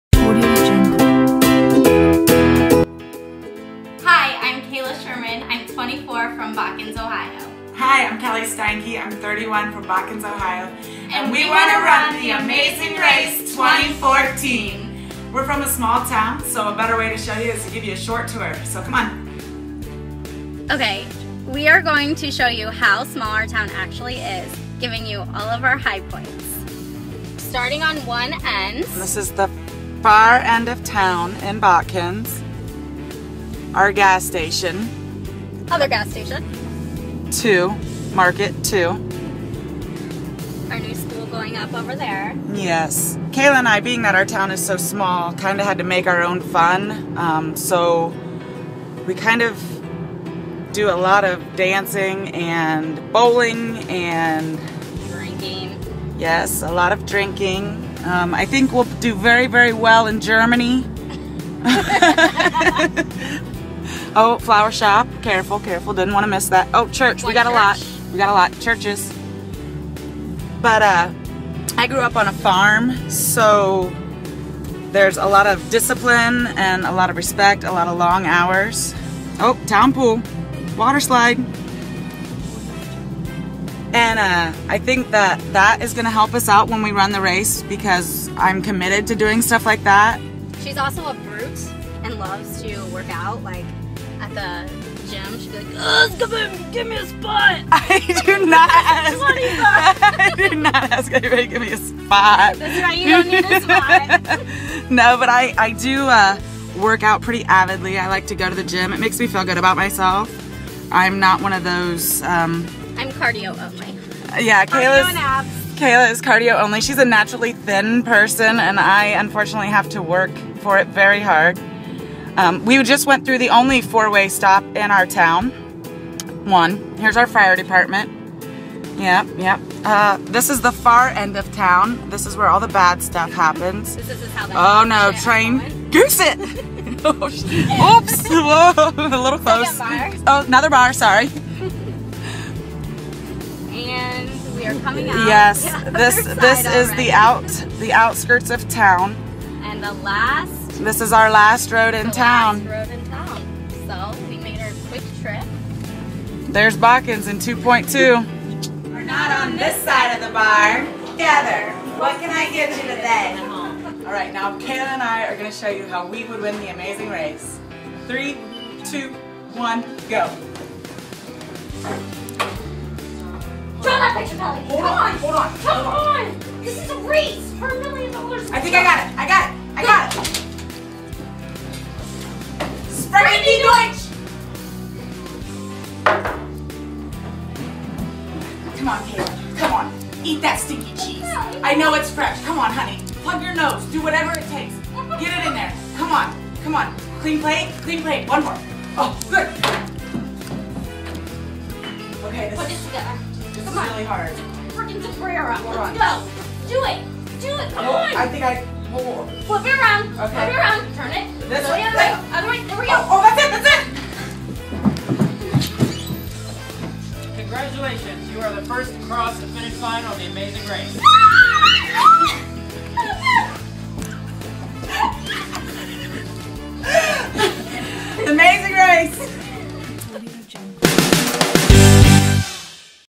Amazing Race 2014 kayla and Kelly from Botkins Ohio
I compiled this video for a friend of the two applicants.  This is the first draft, that being said, if you're reading this, they really like it and don't want me to change it.  Video productions is not my forté, so please curb your comments in that regard.  Also its been optimized for little pocket devices and for cbs' tiny server requirements.  The girls recorded this at a much better quality.  However; if you need any print design... hmu.

also if you like them bombard cbs with demands of choosing them.   tweet away  @AmazingRace_CBS